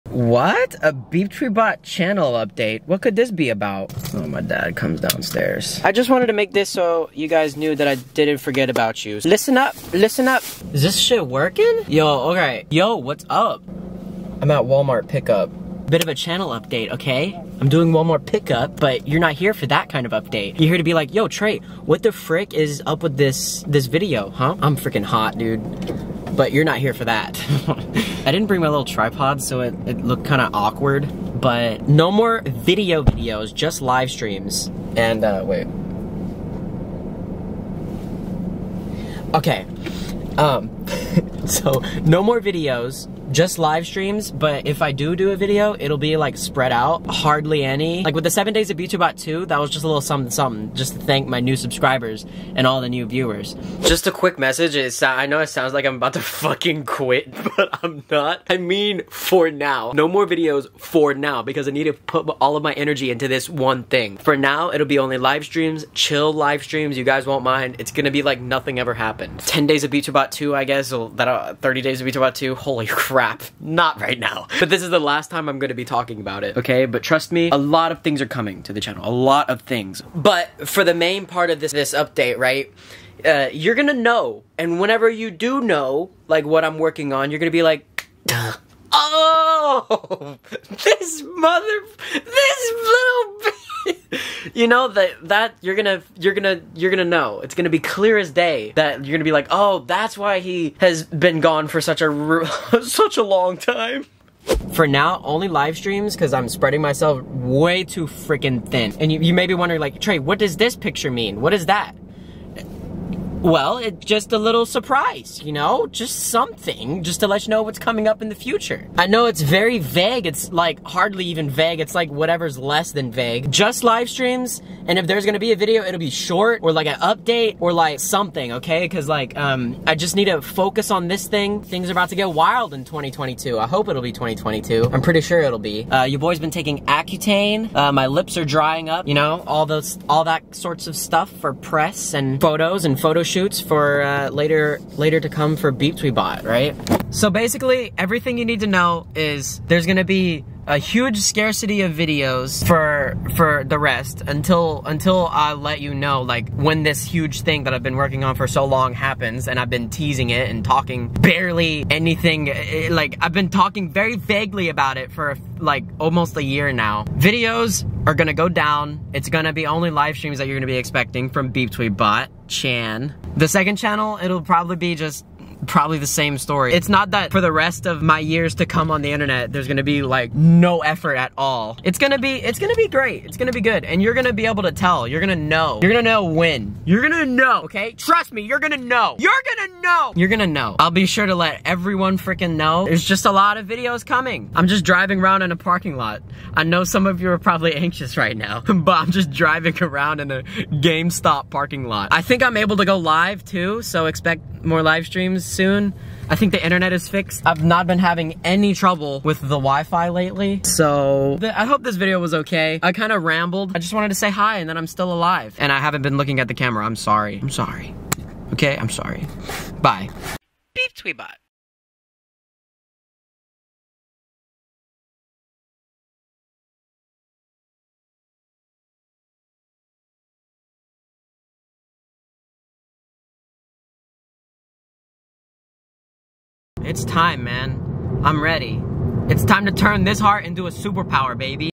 Channel Update Tings Y'all
This doesn't mean I won't post regular videos, I still will it just won't be weekly like normal. they will be much more spread out and not as consistent. in about a year or year and a half's time there will be a lot of special gems for you! For now, be on the lookout for awesome live streams (and a video or 2 lol)!